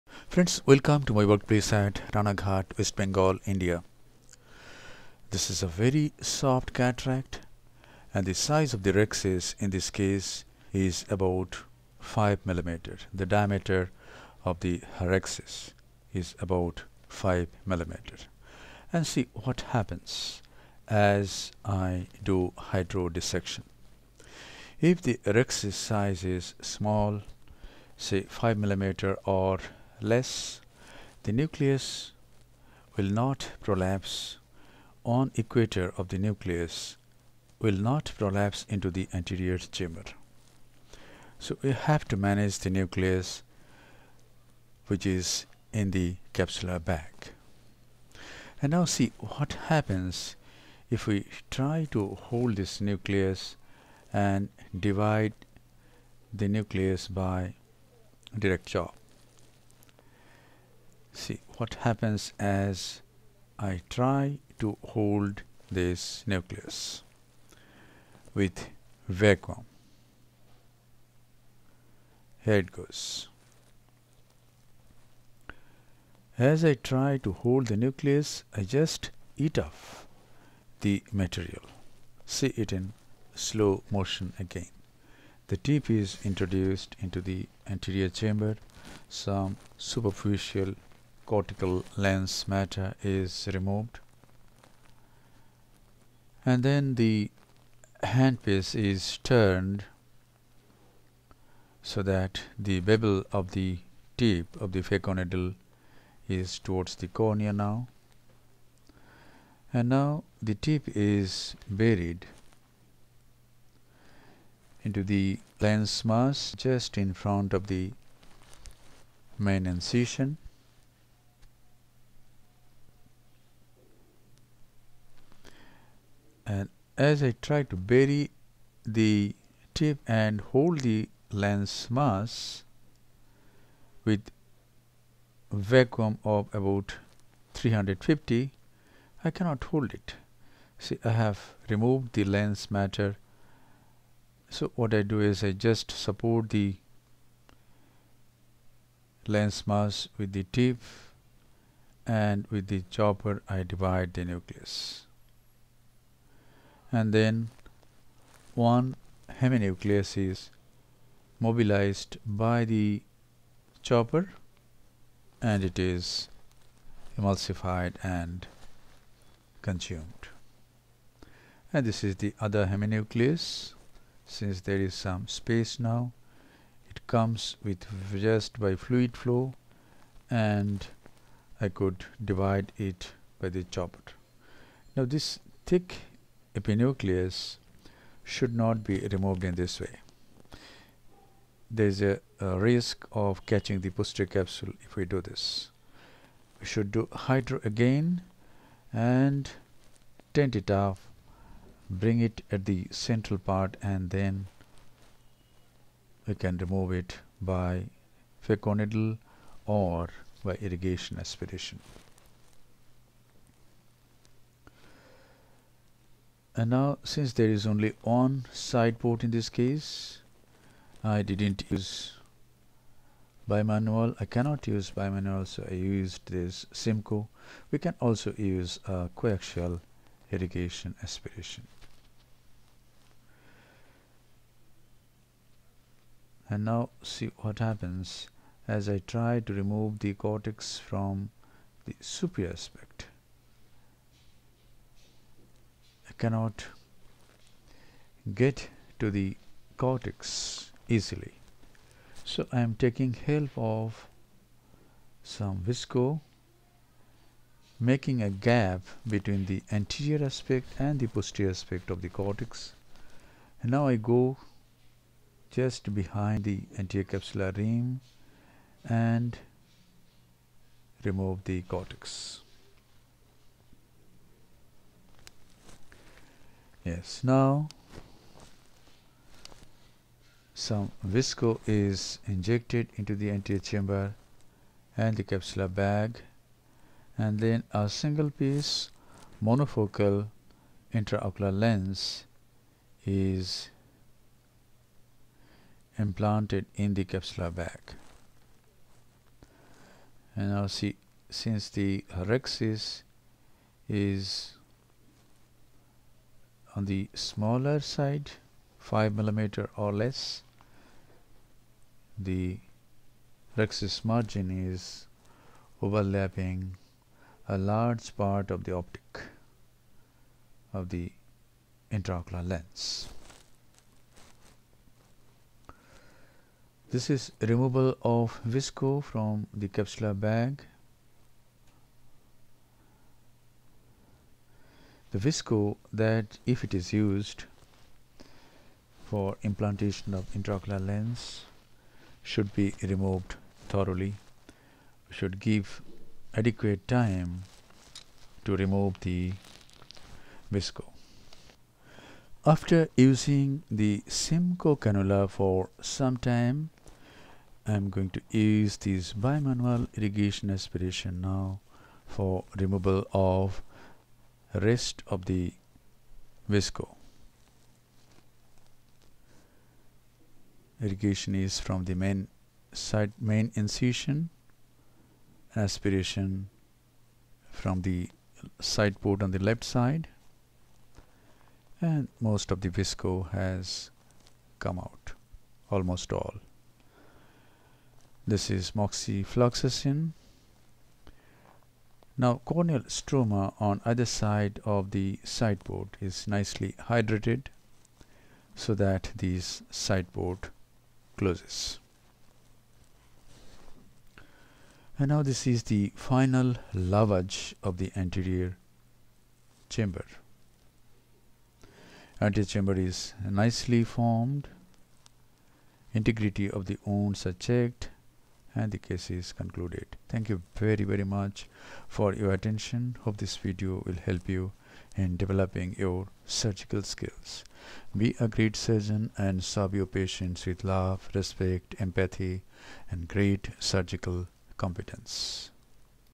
Managing a soft cataract with 5 mm Rhexis : Pradip Mohanta, 14 Oct, 2019
If the size of the Rhexis is small, the nucleus doesn't tilt and the "Lens Tilt Technique" cannot be employed. Direct chop is not possible. We can use either the "Stop and chop" or "Mechanical Chop" technique.